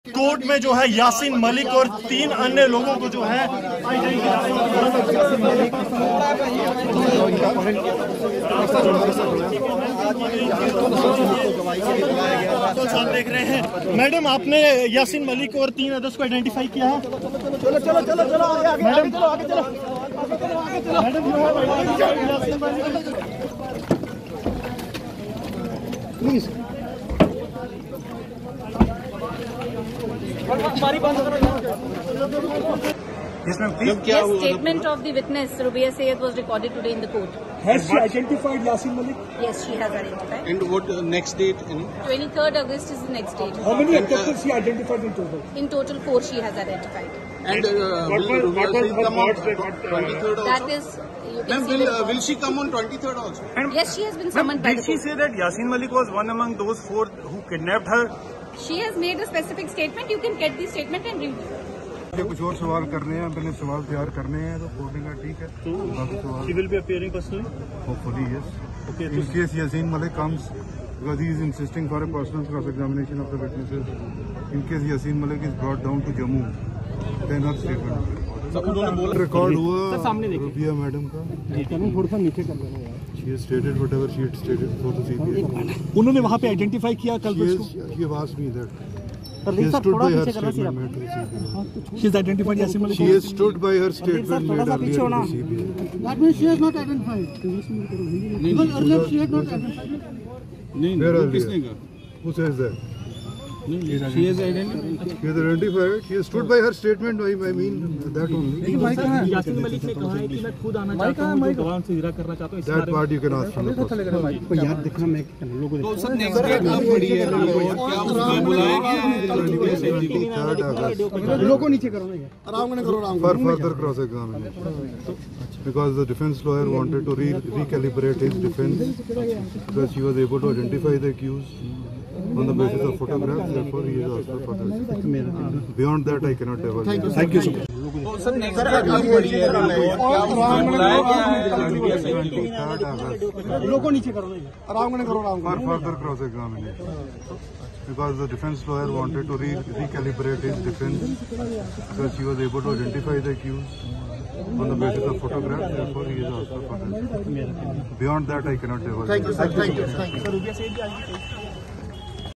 Breaking | Today's hearing of Rubaiya Sayeed case ends, Next date of hearing announced
Breaking | Today's hearing of Rubaiya Sayeed case ends, Next date of hearing